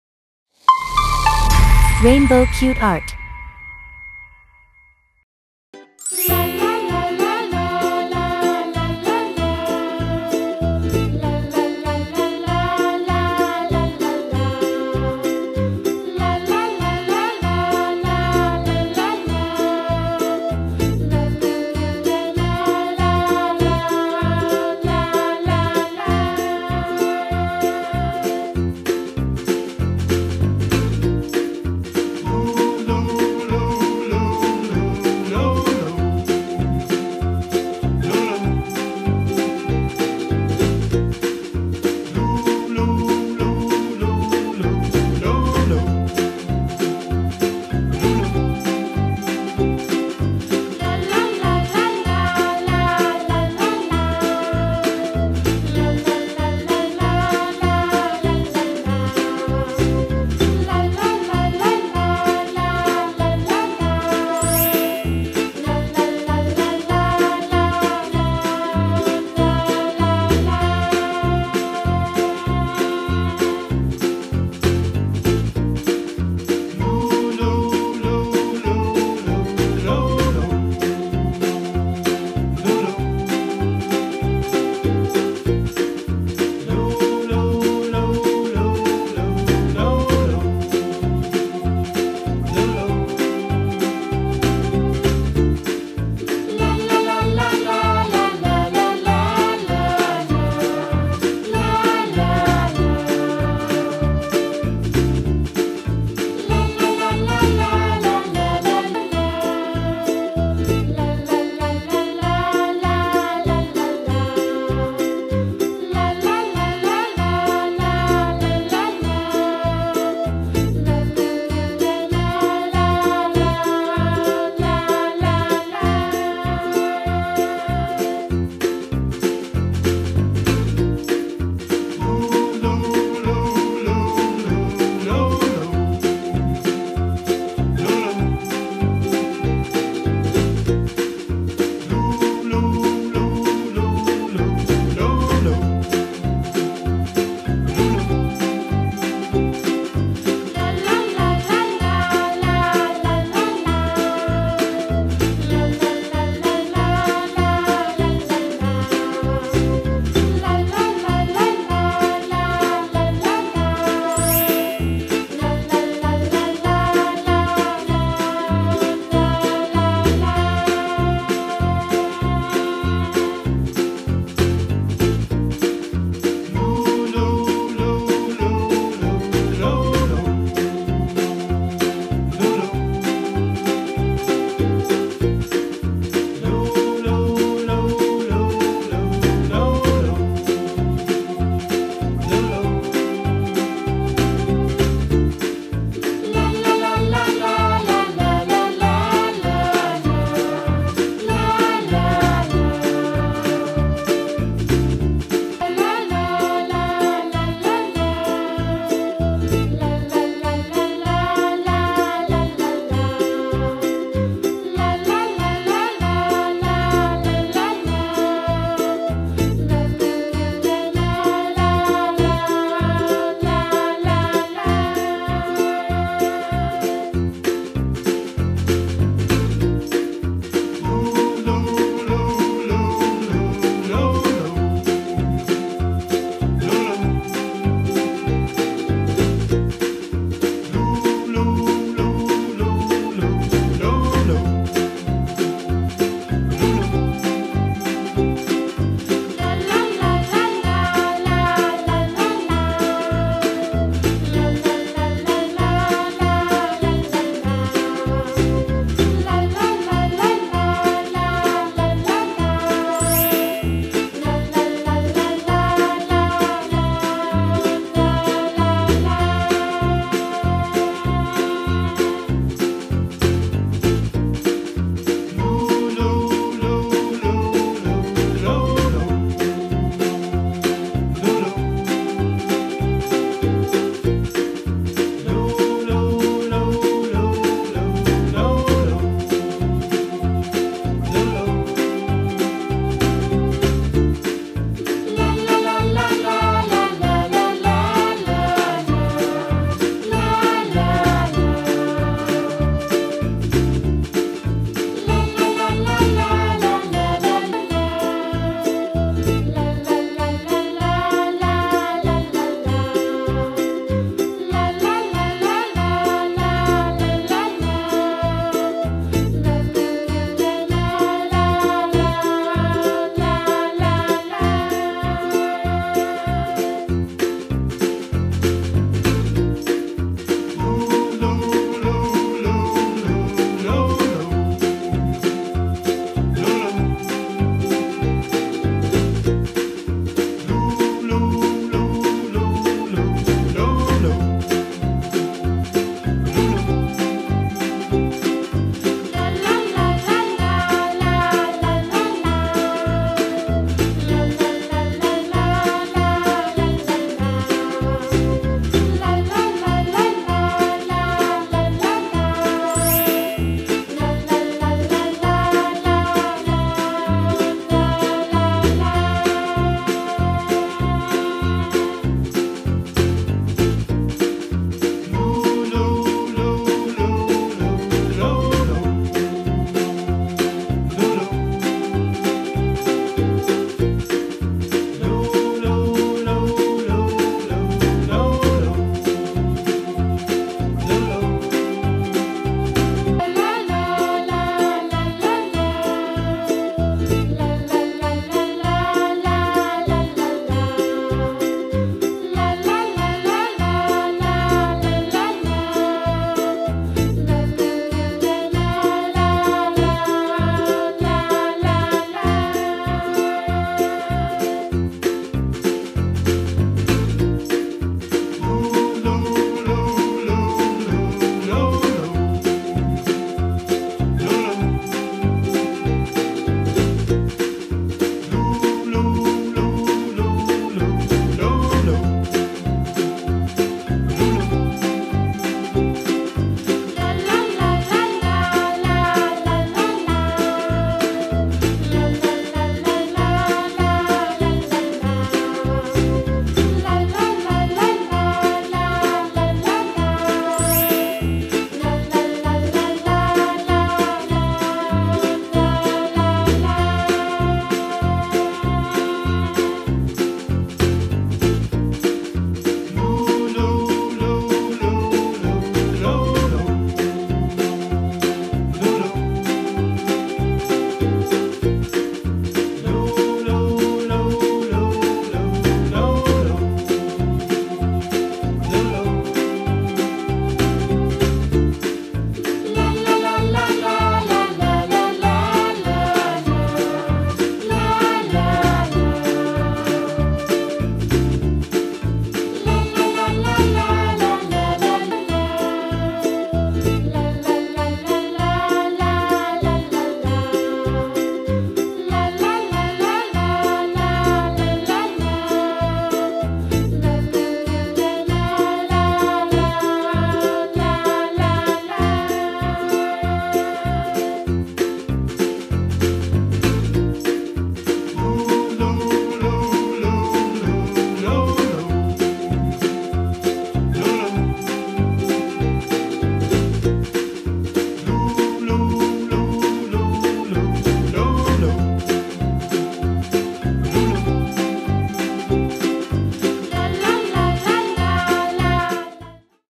How to Draw Shiva Cartoon | Easy Shiva Drawing for Children
Hi Guys! This video I am going to show you Shiva Cartoon Drawing Step by Step Drawing How to Draw Drawing easily, learn sketching & pencil drawing,

We Tested 2 Shiva Drawing Methods Here's What's Best for You!
Is Shiva Drawing REALLY This Easy to Master?